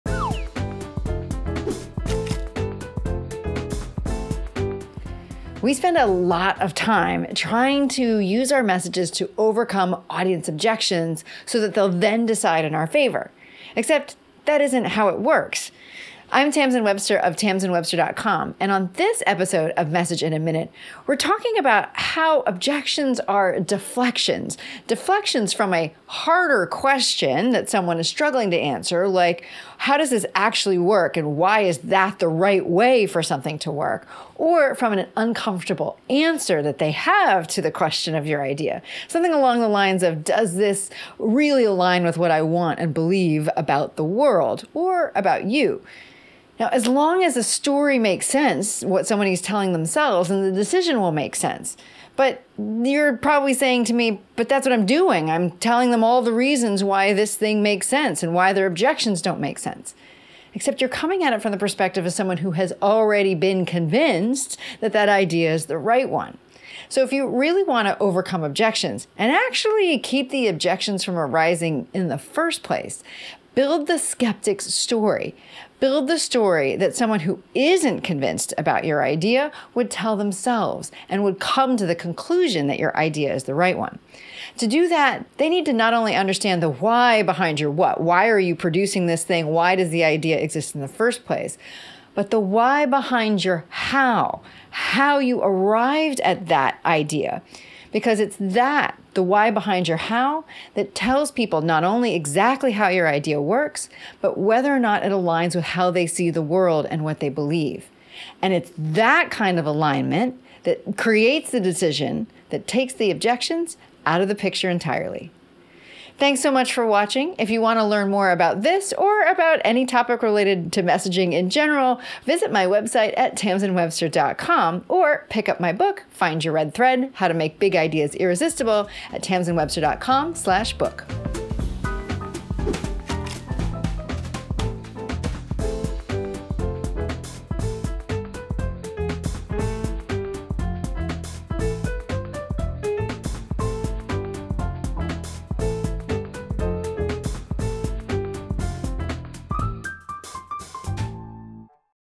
How to Overcome Objections | Message in a Minute
It’s important to remember that when you pose a solution or answer to a problem, you’re coming at it from the perspective of someone who's already been convinced that it’s the right answer. You need to build the skeptic's story! That is, building the story that someone who’s skeptical would tell themselves, explaining the how’s and why’s of what made you arrive at your idea.